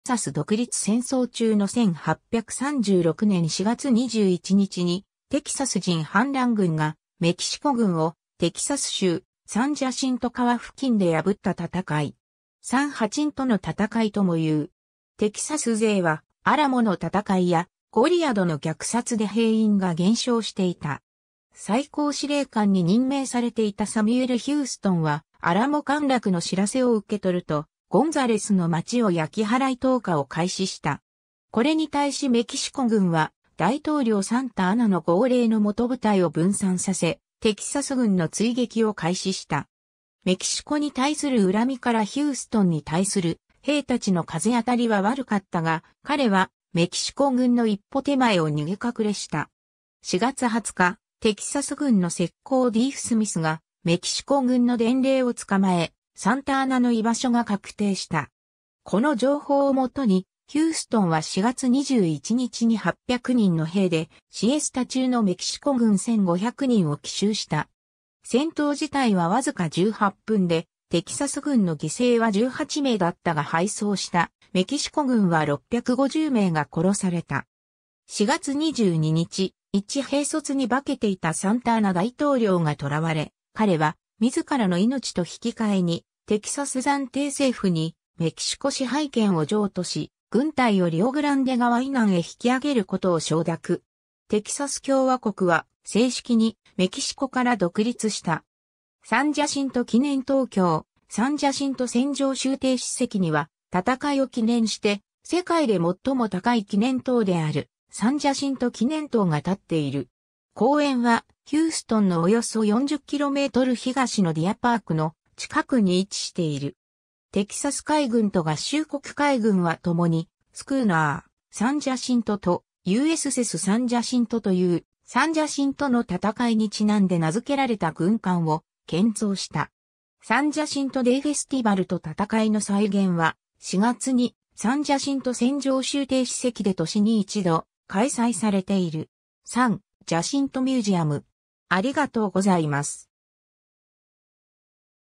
サンジャシントの戦い
アメリカ合衆国の歴史_(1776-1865)
アントニオ・ロペス・デ・サンタ・アナ
サミュエル・ヒューストン
サンジャシントの戦い（サンジャンシントのたたかい）は、テキサス独立戦争中の1836年4月21日に、テキサス人反乱軍がメキシコ軍をテキサス州サンジャシント川付近で破った戦い。
サン・ハチントの戦い（サン・ハチントのたたかい）ともいう。
 テキサス勢は、アラモの戦いやゴリアドの虐殺で兵員が減少していた。
最高司令官に任命されていたサミュエル・ヒューストンは、アラモ陥落の知らせを受け取るとゴンザレスの街を焼き払い東下を開始した。
これに対しメキシコ軍は、大統領サンタ・アナの号令のもと部隊を分散させテキサス軍の追撃を開始した。
メキシコに対する恨みからヒューストンに対する兵たちの風当たりは悪かったが、彼はメキシコ軍の一歩手前を逃げ隠れした。
 4月20日、テキサス軍の斥候ディーフ・スミスがメキシコ軍の伝令を捕まえ、サンタ・アナの居場所が確定した。
この情報を元に、ヒューストンは4月21日に800人の兵でシエスタ中のメキシコ軍1500人を奇襲した。
戦闘自体はわずか18分で、テキサス軍の犠牲は18名だったが敗走したメキシコ軍は650名が殺された。
 4月22日、一兵卒に化けていたサンタ・アナ大統領がとらわれ、彼は自らの命と引き換えにテキサス暫定政府にメキシコ支配権を譲渡し、軍隊をリオ・グランデ川以南へ引き上げることを承諾。
テキサス共和国は正式にメキシコから独立した。
 サンジャシント記念塔 今日、サンジャシント戦場州定史跡には、戦いを記念して、世界で最も高い記念塔であるサンジャシント記念塔が立っている。
公園はヒューストンのおよそ40km（25マイル）東のディアパーク（Deer Park）の近くに位置している。
 テキサス海軍と合衆国海軍は共に、スクーナー「サンジャシント」と「USS サンジャシント」（サンジャシントを参照）という、サンジャシントの戦いにちなんで名付けられた軍艦を建造した。
 サンジャシント・デイ・フェスティバルと戦いの再現は、4月にサンジャシント戦場州定史跡で年に一度開催されている。
 ^ San Jacinto Museum